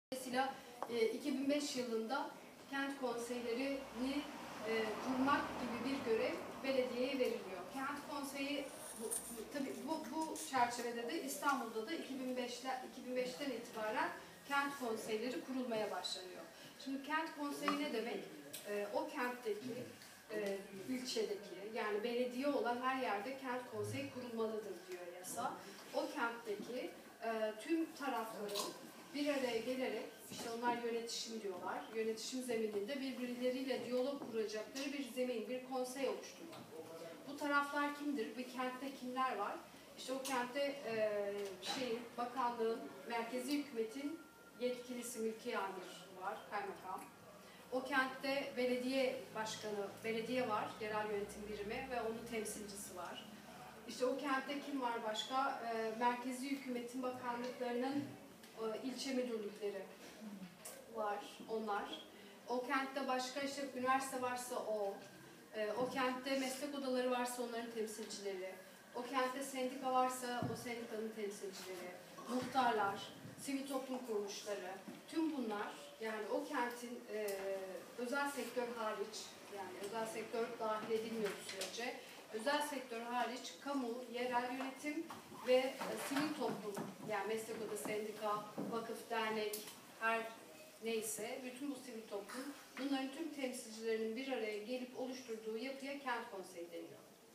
İkbal Polat anlatıyor - Kent Konseyi nedir?
İkbal Polat kent konseyleri ile ilgili deneyimleriyle uzman görüşlerini ve bilgilerini paylaşıyor...